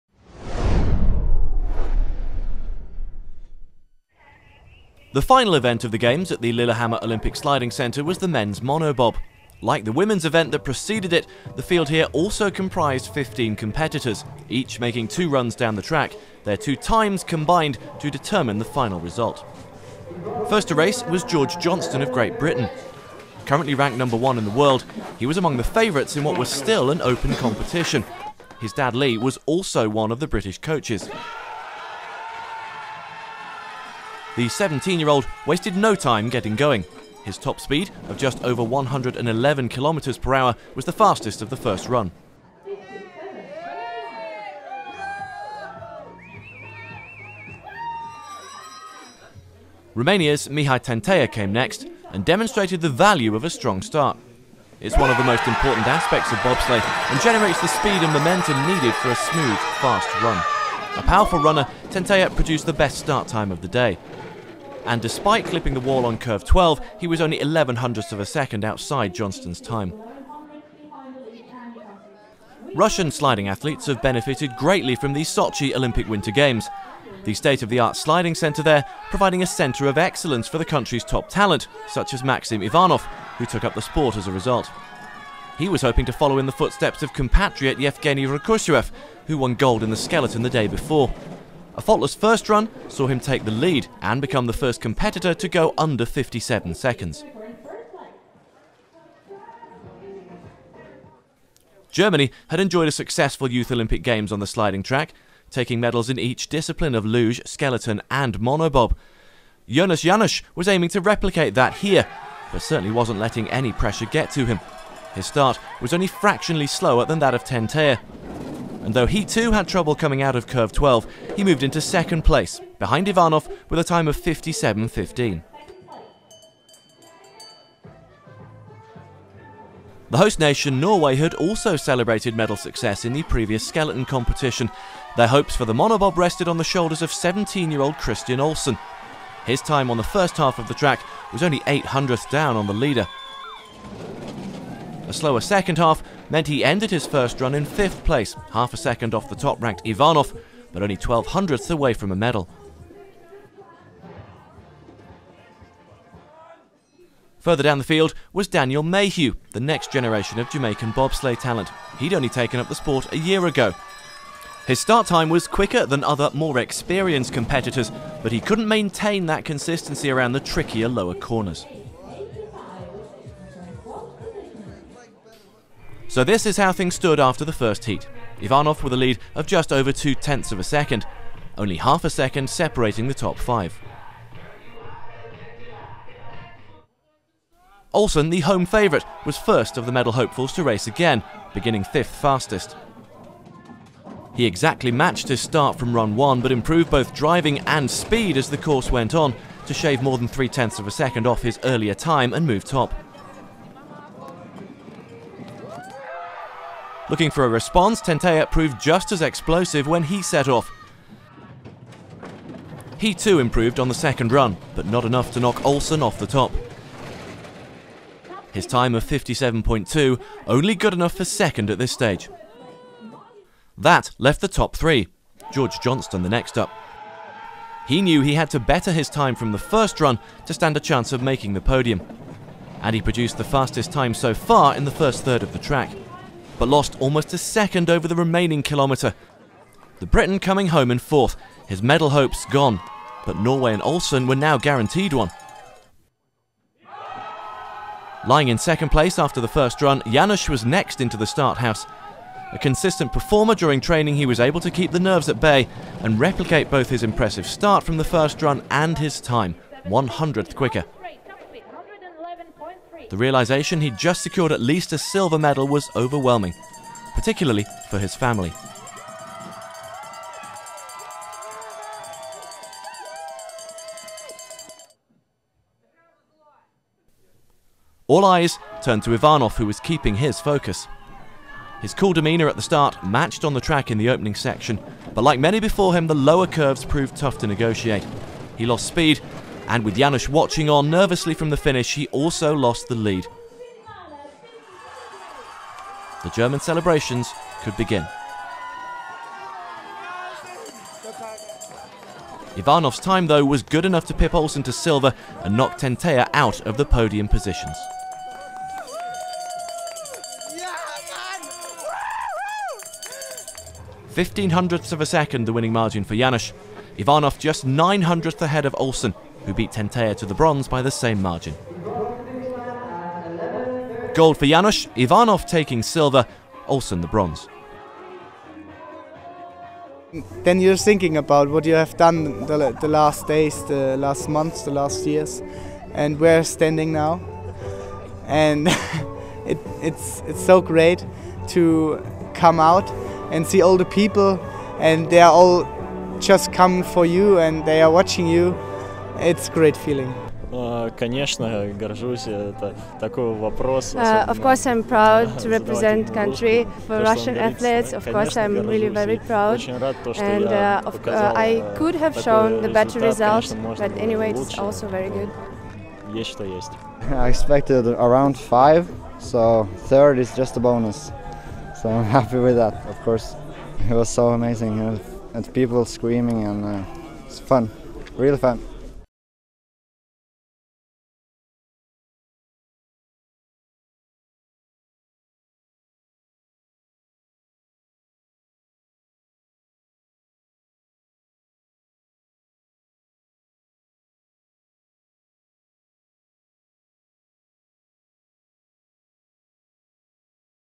Monobob - Jonas Jannusch (GER) wins Men's gold | Lillehammer 2016 Youth Olympic Games
Highlights of the Men's Monobob from the Lillehammer Olympic Sliding Centre on Day 9 of the 2016 Youth Olympic Games in Lillehammer.

Germany won their second Bobsleigh gold medal of Lillehammer 2016 after Jonas Jannusch won the Men's Monobob.

Medal Winners:

Gold: Jonas Jannusch (GER)
Silver: Maksim Ivanov (RUS)
Bronze: Kristian Olsen (NOR)